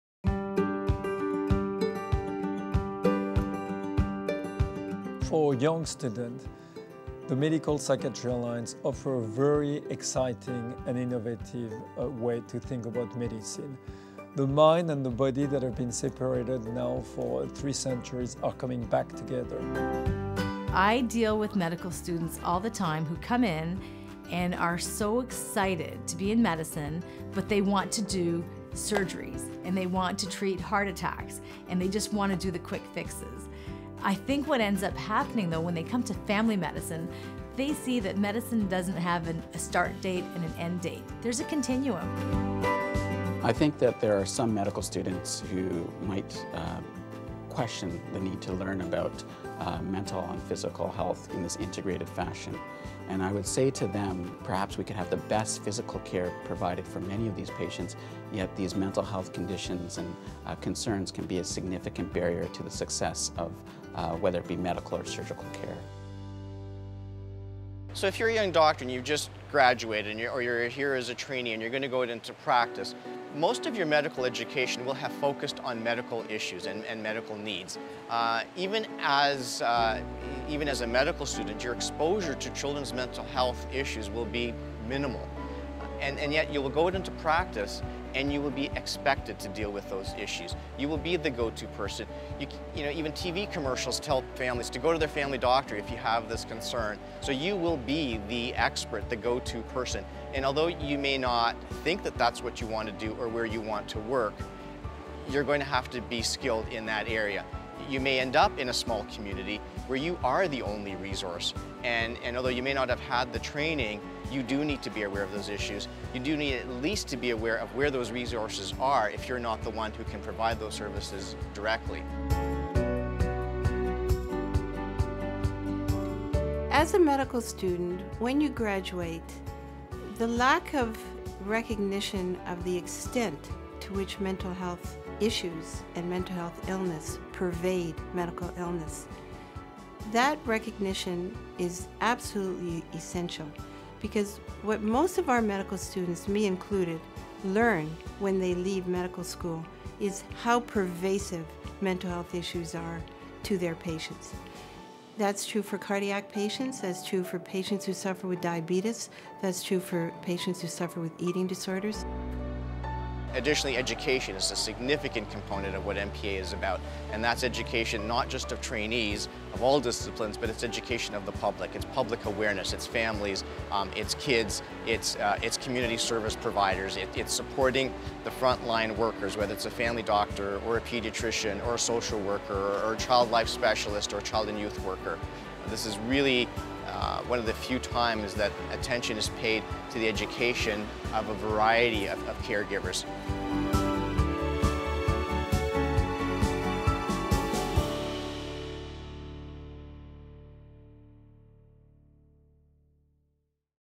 Medical Psychiatry Alliance: Transforming medical education and training
The Medical Psychiatry Alliance is addressing gaps in our health care system from several fronts – through better access and evidence-based clinical care, as well as through education and training. We need to change the way we train health care professionals across all disciplines, so that they are better equipped with the resources and tools they need to screen and diagnose patients living with combined mental and physical illnesses.